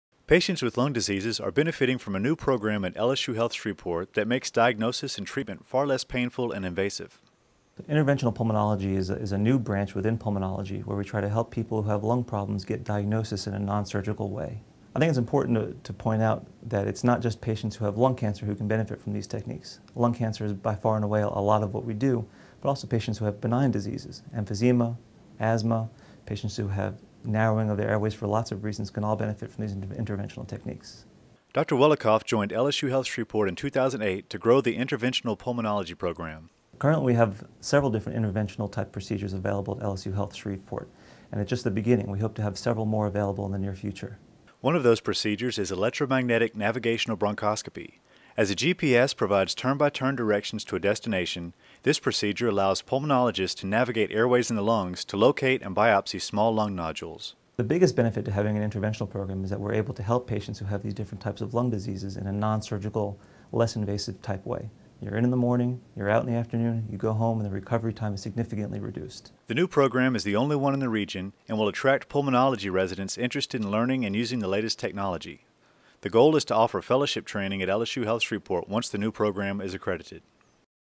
Healthy U - Interventional Pulmonology.m4v
LSU Health Shreveport has a new program that helps patients with lung diseases.